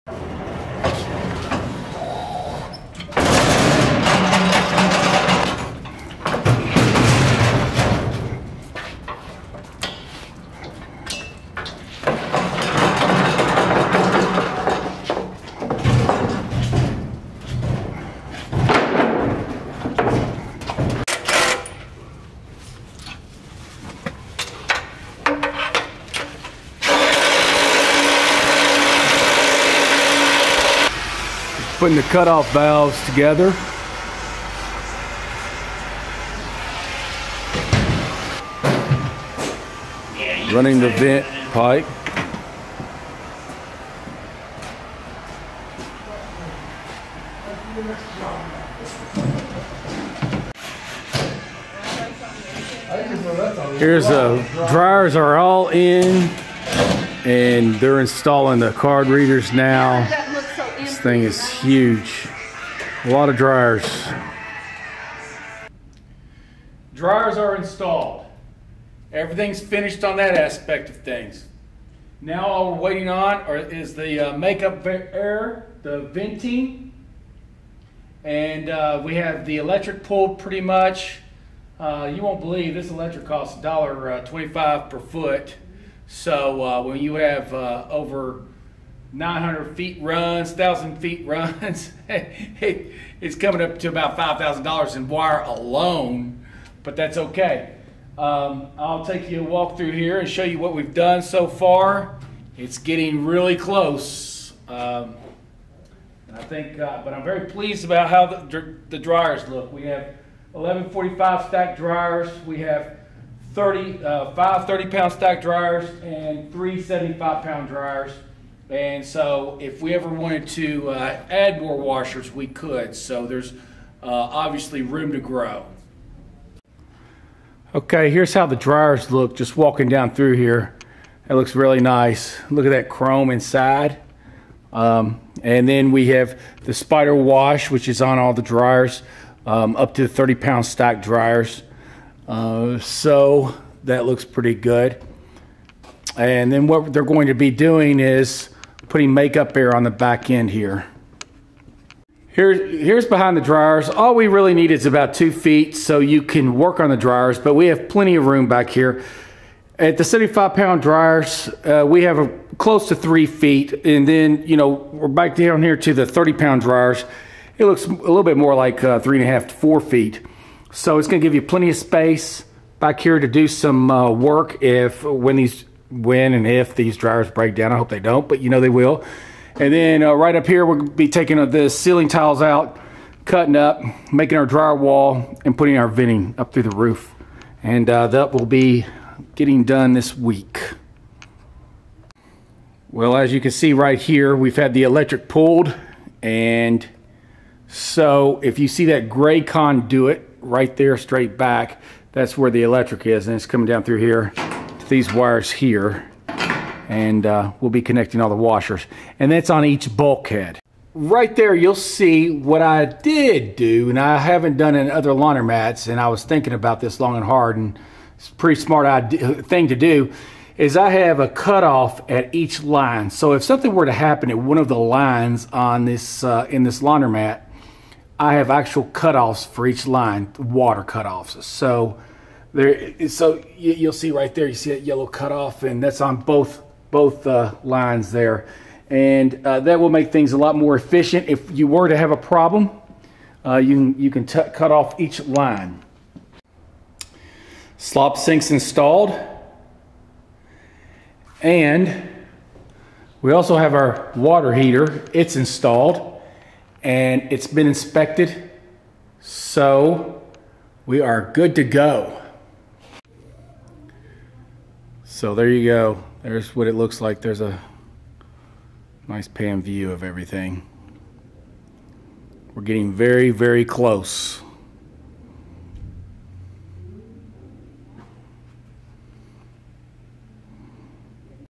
How to build a laundromat part 7 1080p
It's getting pretty close to being finished.  Here are some sights and sounds of a laundromat buildout.